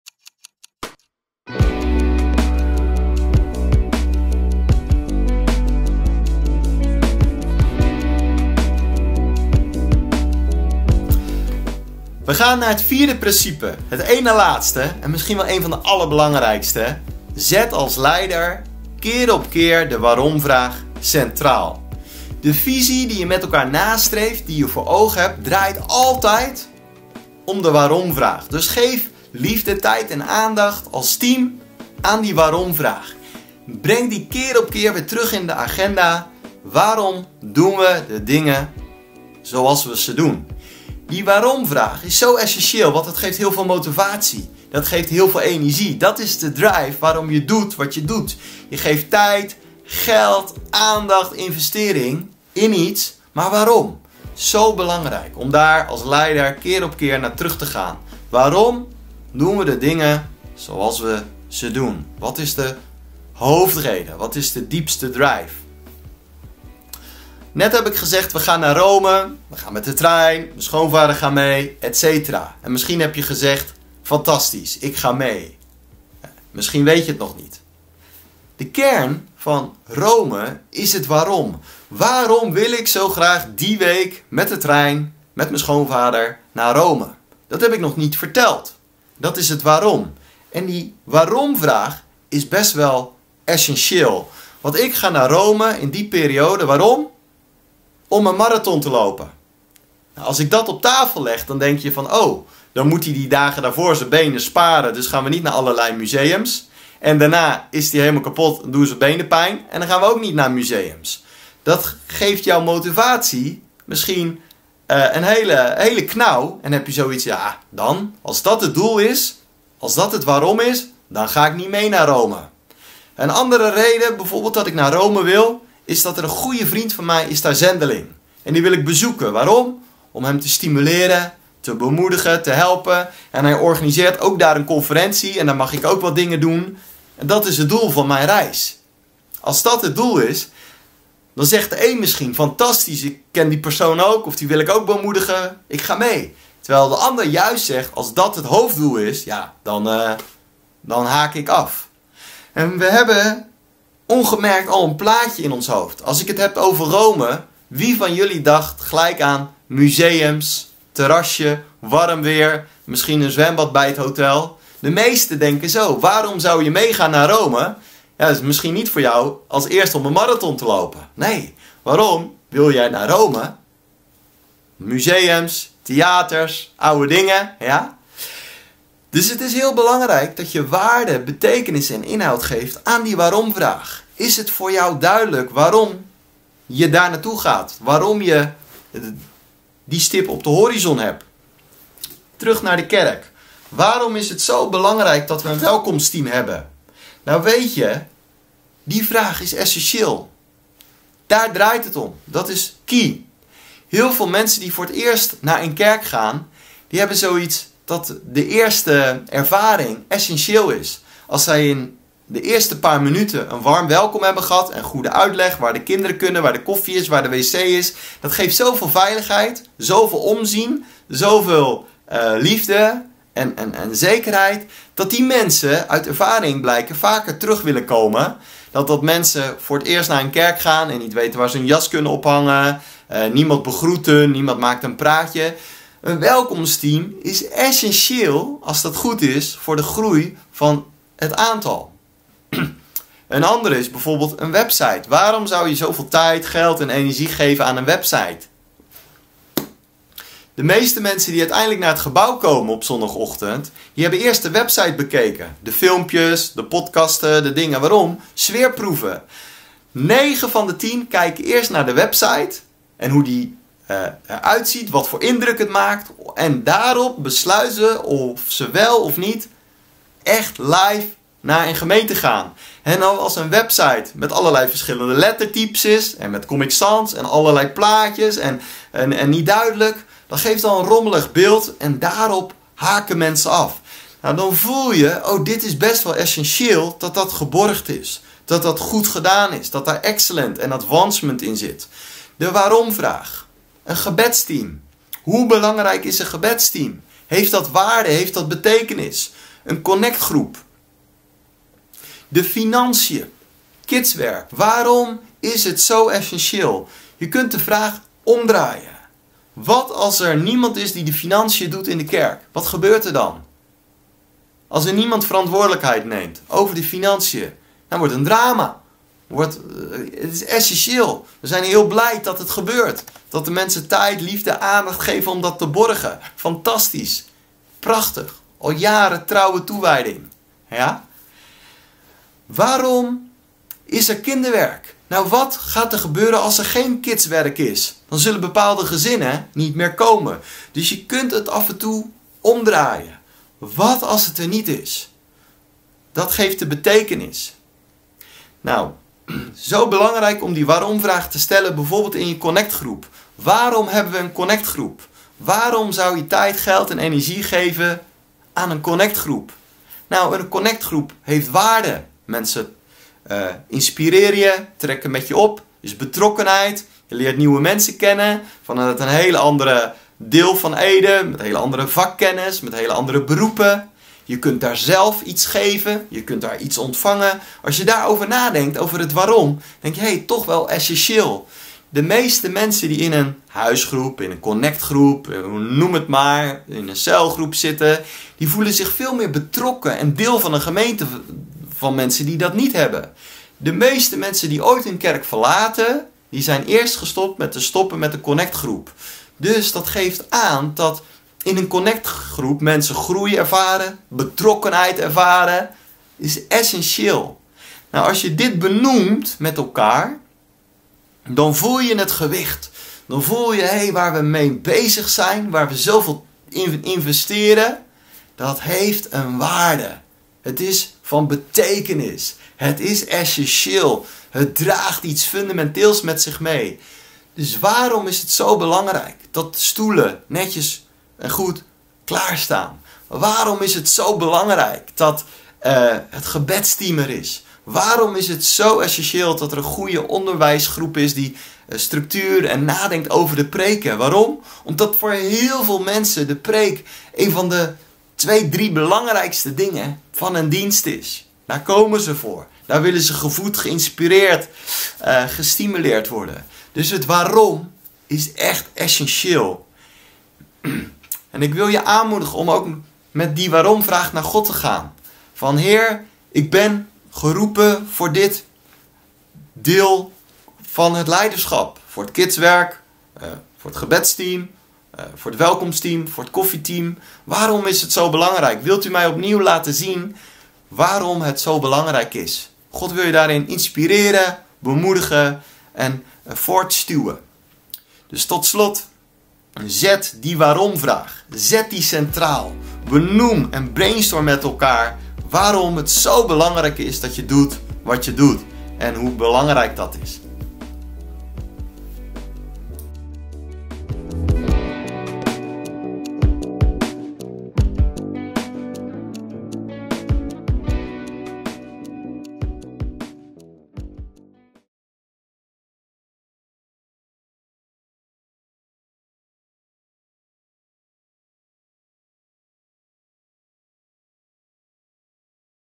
Overtuigde visionair 4/5 - ConnectCollege Leiderschap
Een terugblik op avond 1, Een leider als overtuigde visionair.
Principe 4: Zet het waarom keer op keer centraal.

In deze clip kijkt Corstian terug op de eerste ConnectCollege avond over gezond leiderschap. Het thema van deze avond was 'Een leider als overtuigde visionair', en dan met name de 1e van de 5 gedeelde principes, 'Een leider ziet het al voordat iemand anders het ziet'.

Er zijn 5 korte clips over de volgende principes:
1- Een leider ziet het al voordat iemand anders het ziet
2- Een leider geeft een duidelijk beeld van het beloofde land
3- Communiceer duidelijk over de weg naar gezamenlijke bestemming
4- Zet het waarom keer op keer centraal
5- Voldoe aan de voorwaarden om iemand in beweging te kunnen krijgen voor de visie